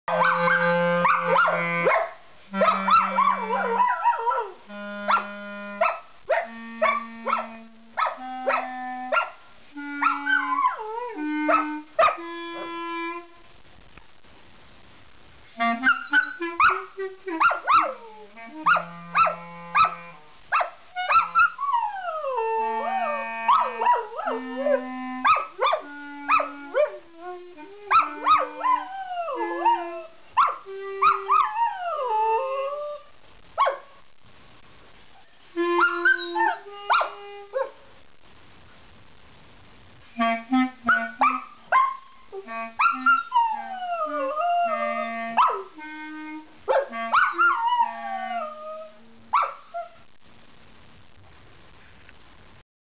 Dog Sings The Blues
Ruby, a standard poodle, sings the blues during a clarinet lesson